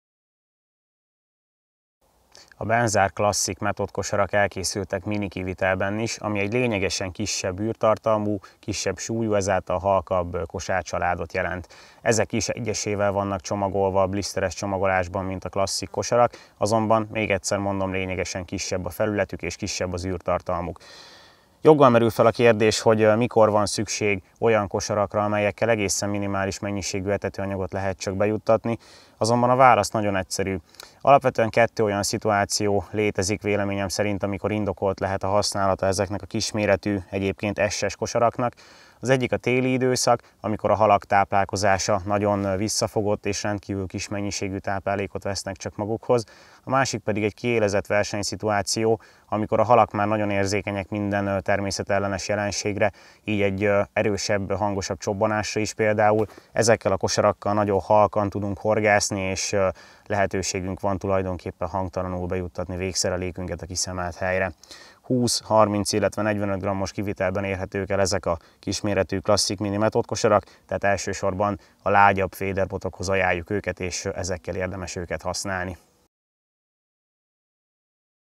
Benzár Classic Mini Method kosár
A method módszer lelke sokak szerint a megfelelően elkészített etetőanyagban rejlik. Ebben igazuk is van, de a végszerelék részét képző kosárra is érdemes figyelmet fordítani.

Az új Benzár Classic Mini method kosár akár a partközeli vagy kereső feederes pecákhoz is tökéletes alternatívát nyújt, méretskálájának köszönhetően.

Bármely, a method technikával alkalmazott és közkedvelt csalihoz tökéletes megoldás, legyen szó waftersről, popupról vagy bármilyen süllyedő csaliról, sőt a legújabb Expanda pelletekkel is tökéletes összhangot képvisel.

A Classic Mini mérete miatt, akkor is megfelelő alternatívát jelent, mikor a halak finoman vagy alig táplálkoznak és azokat csak egy kis mennyiségű, de prémium etetőanyaggal lehet a csali közelébe terelni. A ponty mellett, keszeg-kárász és kisebb halak horgászatához is jó opció.

Áramvonalas kialakítása rendkívül jó dobhatóságot eredményez, kis mérete ellenére is.

Versenykörülményekhez is kifejezetten ajánljuk a szériát, de a hétköznapi pecák alkalmával sem fognak csalódást okozni.